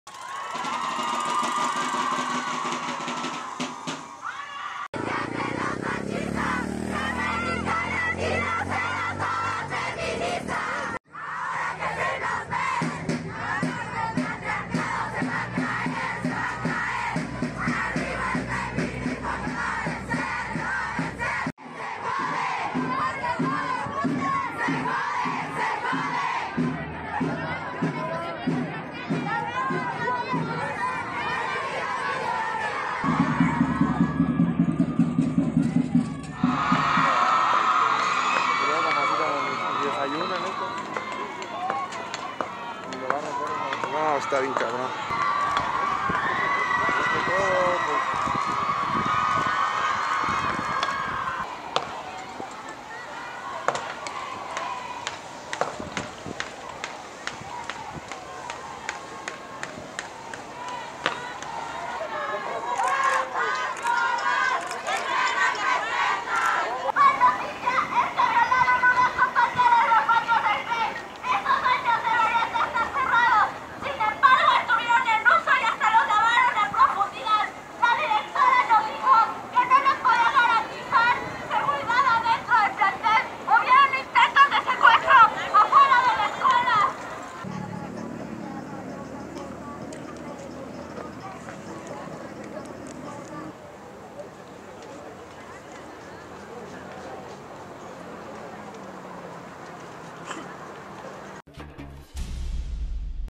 Marchan del CCH Sur a Rectoría por violación de estudiante; demandan más seguridad
Estudiantes y colectivas feministas marcharon del CCH Sur a Rectoría, demandaron mayores medidas de seguridad. La semana pasada, una alumna de esa escuela fue violada en el interior del plantel. Info